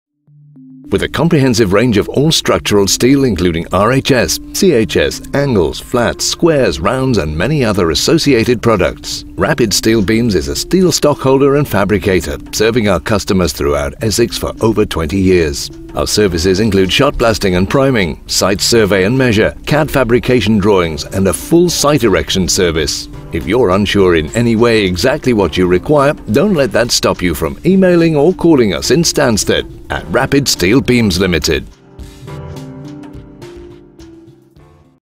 Steel Stockholders - Rapid Steel Beams Ltd
Do you need steel for a building project?  Do you need to prepare metal for painting? Rapid Steel Beams Ltd are steel stockholders, providing shot blasting & priming with a full site erection service